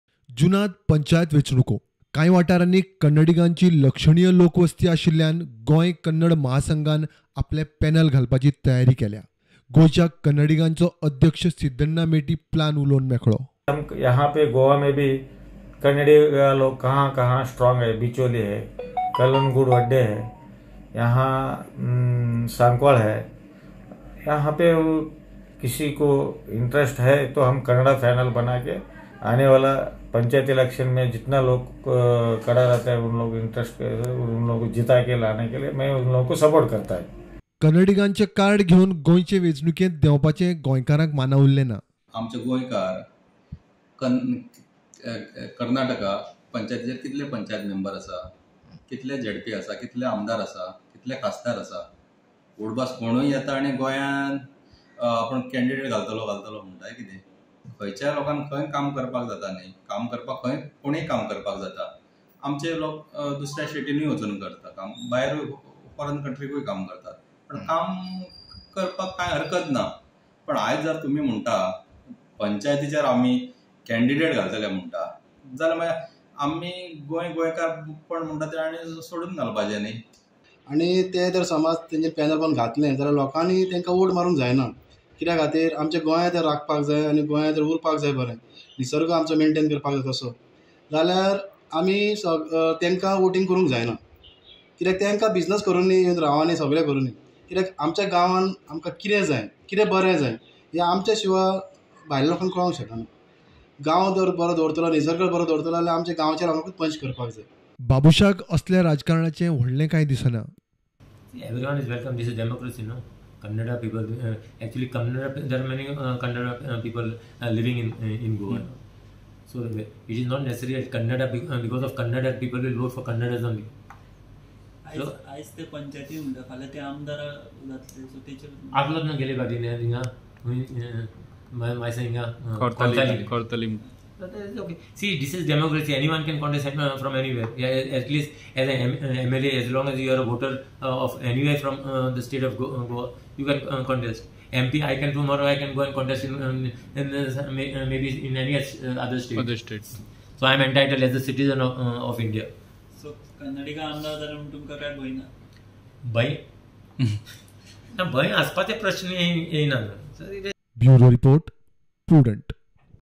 SEE WHAT BABUSH HAD TO SAY ON KANNADA MAHASANGH’S PLAN TO FLOAT PANELS FOR PANCHAYAT POLLS
All Goa Kannada Mahasangh Honorary President Siddhana Meti has said that their organization will float a panel of candidates for the upcoming Panchayat elections in Goa.
Reacting to Goa Kannada Mahasangh’s statement, Minister Babush Monserrate said “ Everyone is welcome, this is democracy. Anyone can contest from anywhere, as long as you are a voter from the state of Goa. For MP elections, I can go and contest in any other state,”
Salvador do Mundo panch member  Sandeep Salgaonkar has said that Kannda people should only stick to doing work, and not come into politics in Goa..